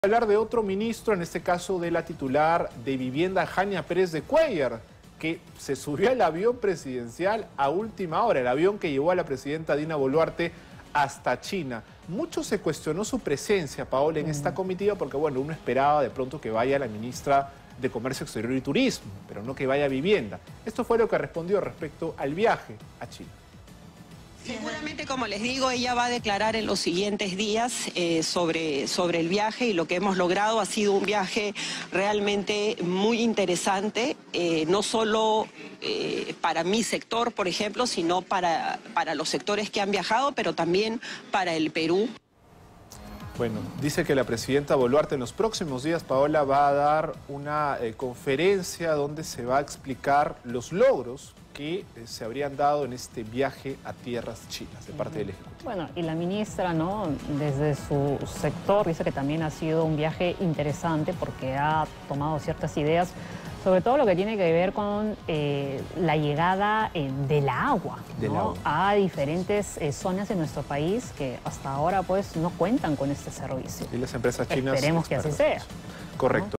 Ministra de Vivienda tras viaje a China: “En los próximos días la presidenta hablará con la prensa”
La ministra de Vivienda, Construcción y Saneamiento, Hania Pérez de Cuéllar, se refirió al reciente viaje que realizó junto a la comitiva de la presidenta Dina Boluarte a China.
Según Informó a la prensa, en los próximos días, la jefa de Estado ofrecerá una conferencia a los medios para detallar los logros alcanzados durante esta visita al país asiático.
Asimismo, destacó que durante su estancia en China se han alcanzado acuerdos significativos en beneficio del país. "Los resultados de este viaje han sido sumamente positivos no solo para mi sector, sino para todos los sectores involucrados y para el Perú en su conjunto", subrayó.
CUESTIONAMIENTOS POR SU VIAJE
La inclusión de la ministra Hania Pérez de Cuéllar en la comitiva presidencial para este viaje a China se produjo en el último momento, lo cual generó amplios cuestionamientos en su sector, especialmente en un momento en el que gran parte de la población enfrenta dificultades significativas de acceso al agua potable.